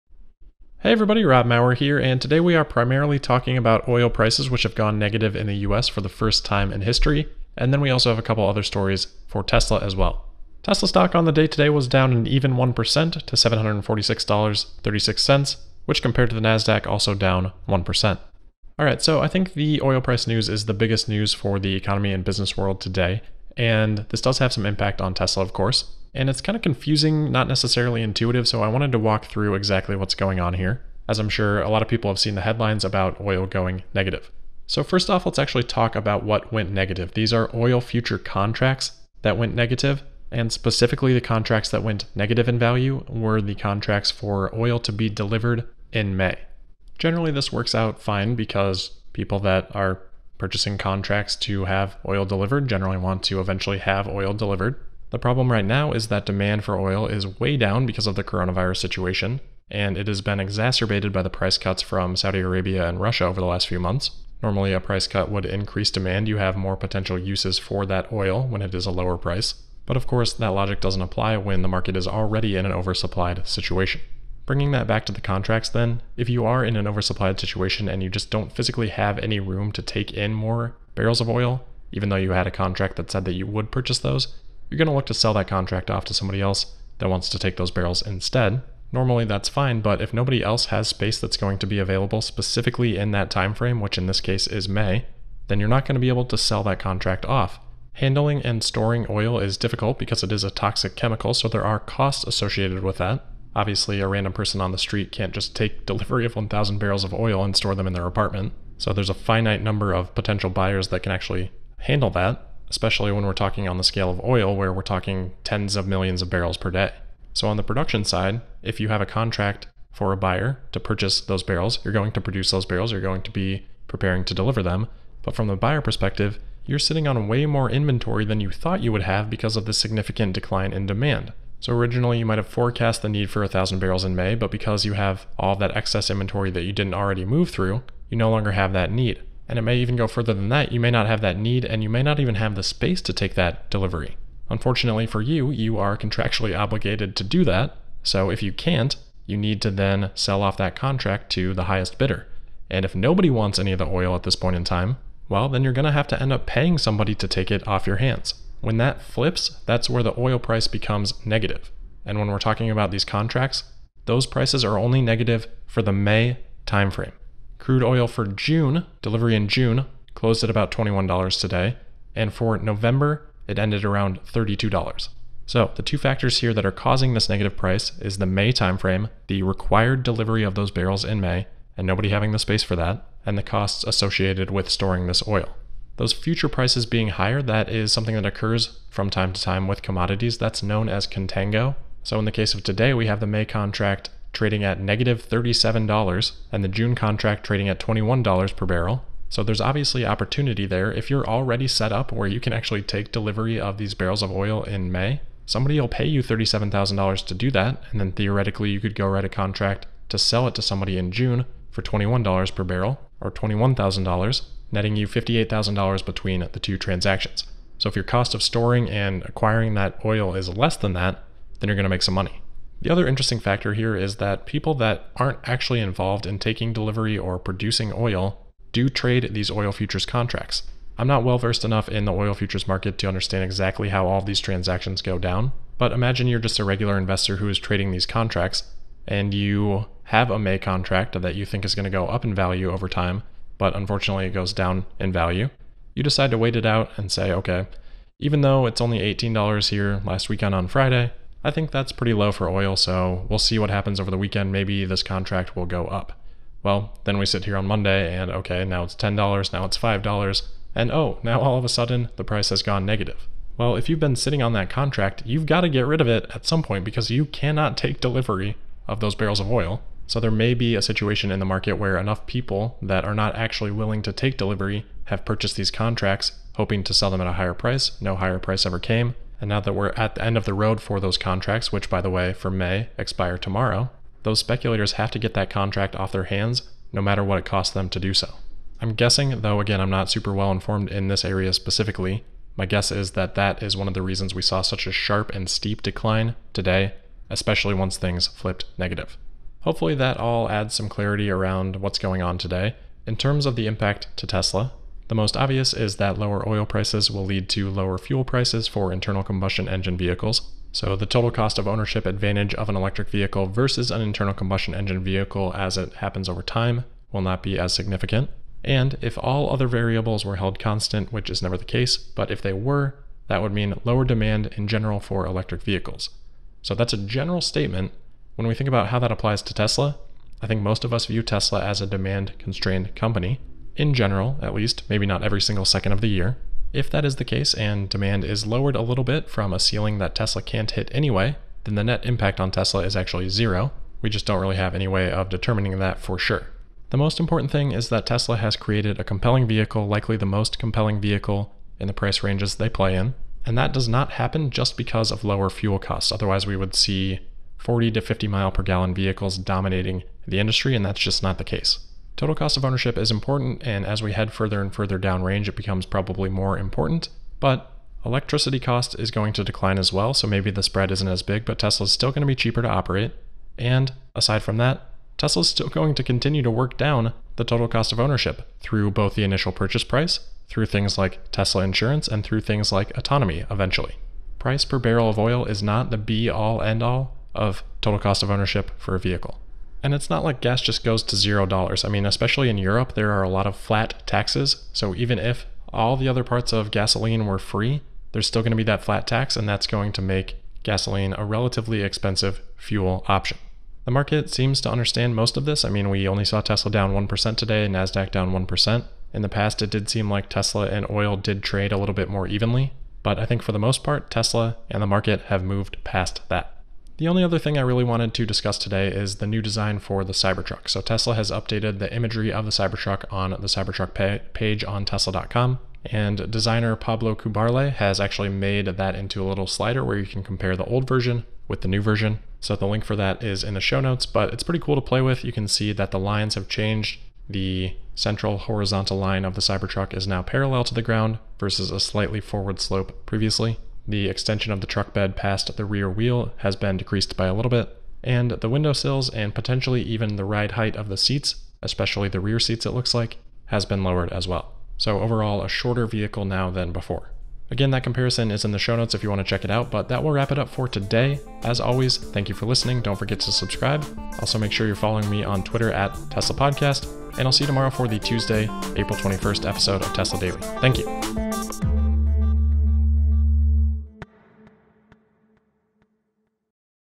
How Can Oil Prices Turn Negative and How is Tesla (TSLA) Impacted? + Cybertruck Redesign
➤ Oil contracts turn negative for the first time in history
➤ How can oil prices turn negative and how are Tesla and TSLA stock impacted?